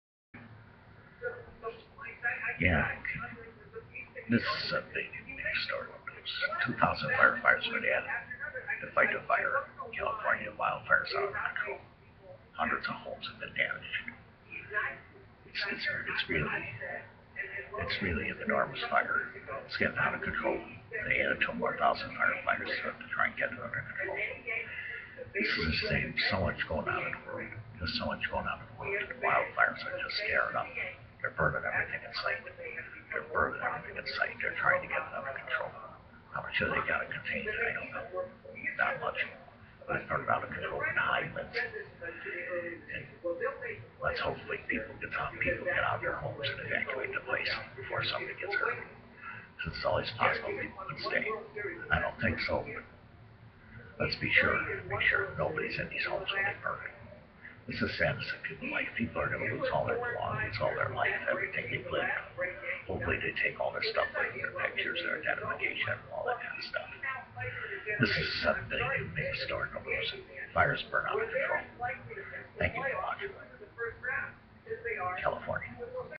RE;King fire: 2000 firefighters added to fight massive wildfire as winds expected
King fire: 2000 firefighters added to fight massive wildfire as winds expected